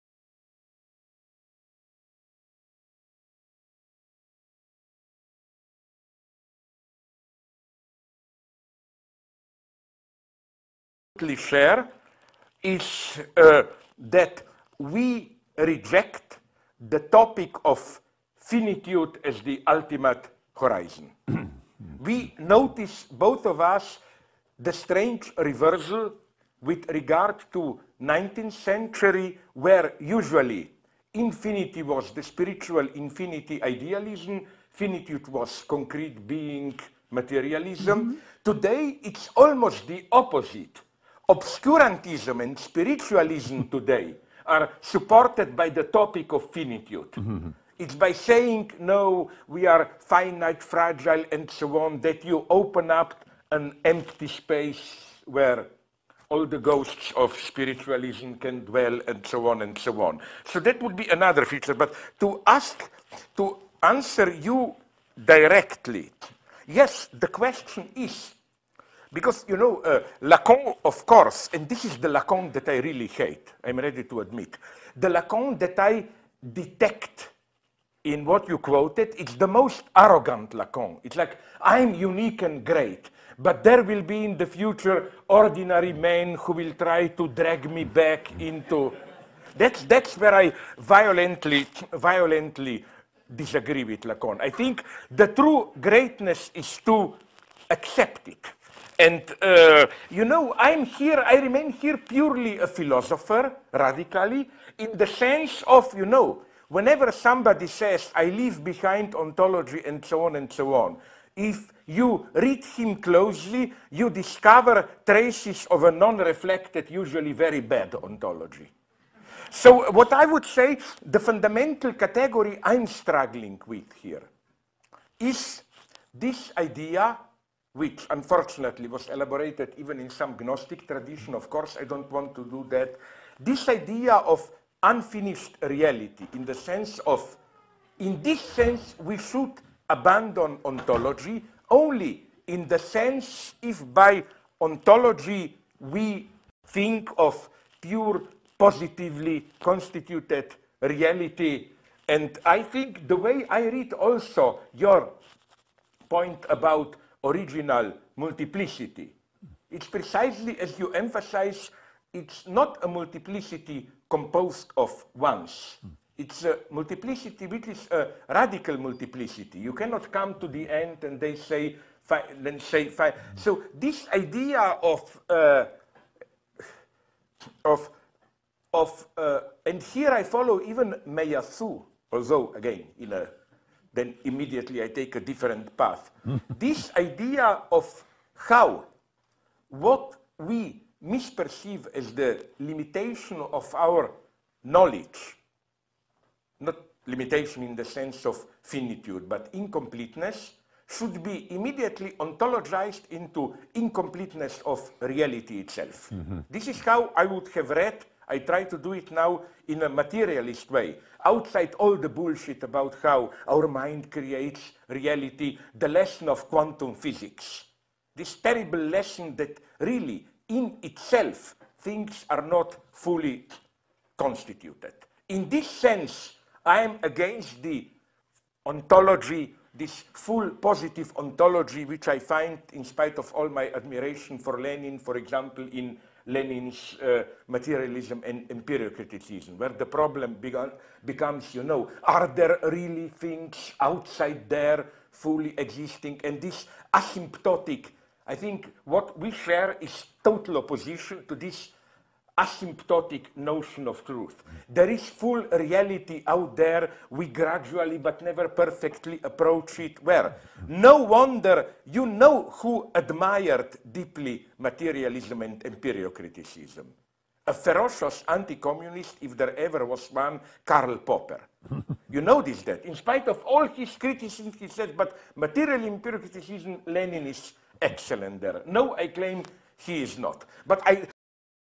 Slavoj Žižek against Finitism
Slavoj Žižek emphasize  his  position about on the opinion of lenin and Meillassoux' finitism